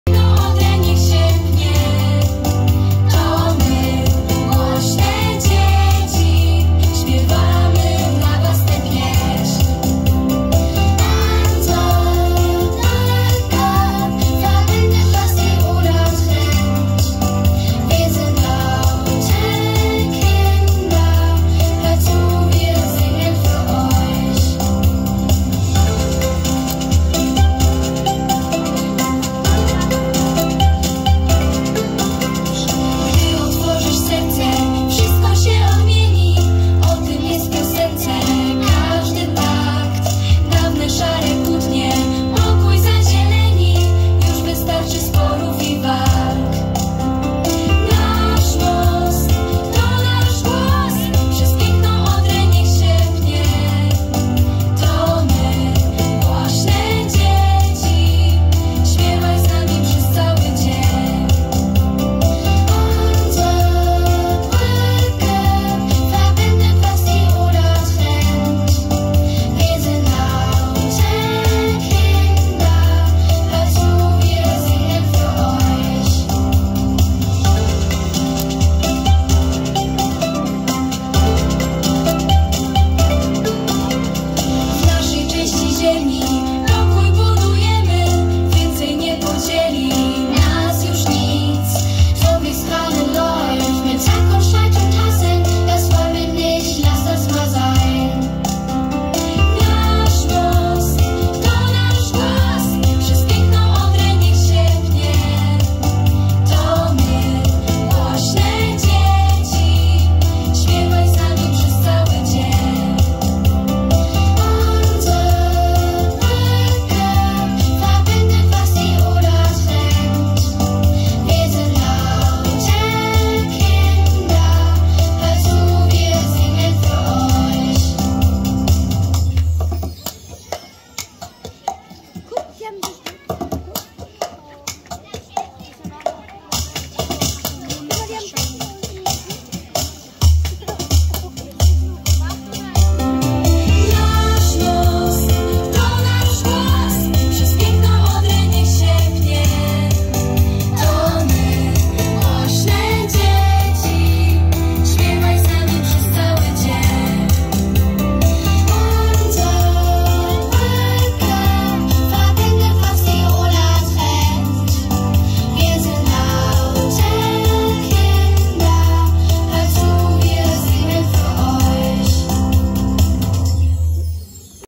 Inauguracja Miejskiego Święta Hanzy. Polsko-niemiecka piosenka
Inauguracja Miejskiego Święta Hanzy 2023 rozpoczęła się od polsko-niemieckiej piosenki wykonanej wspólnie przez dzieci z obu miast.